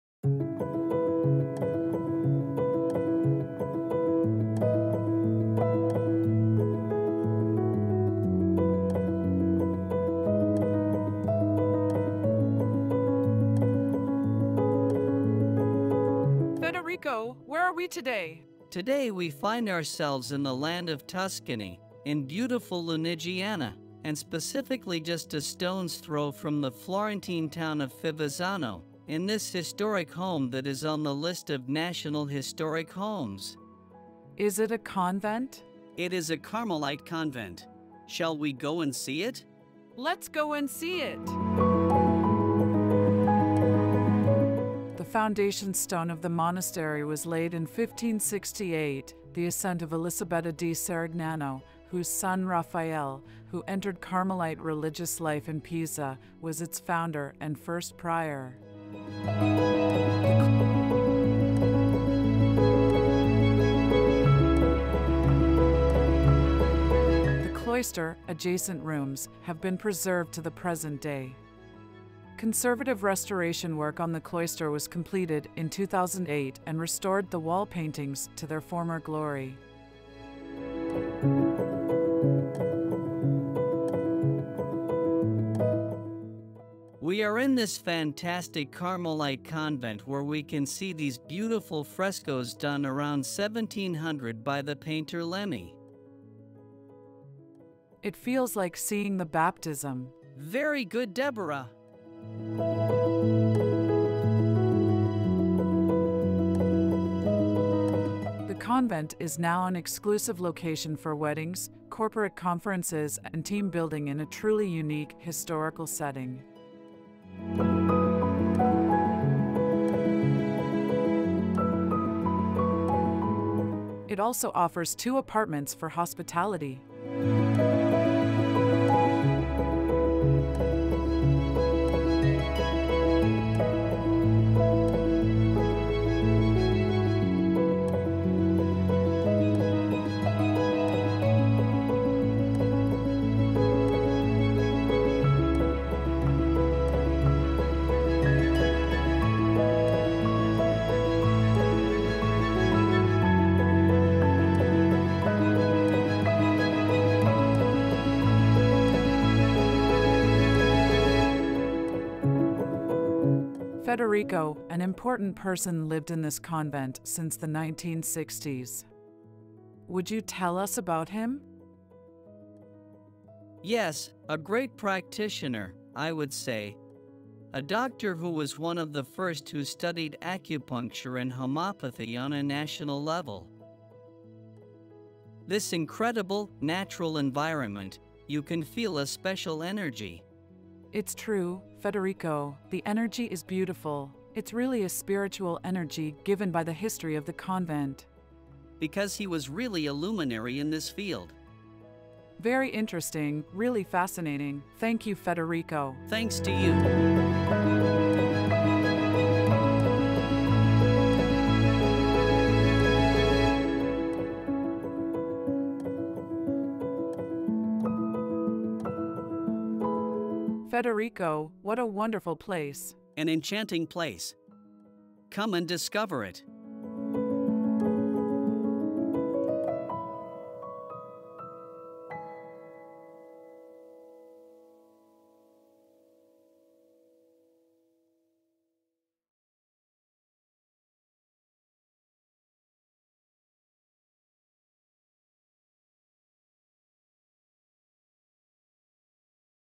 Ancient convent in Tuscany for sale
A place of charm and history in beautiful Lunigiana. An ancient convent dating back to 1568 is now a location for events and team building. Come and discover it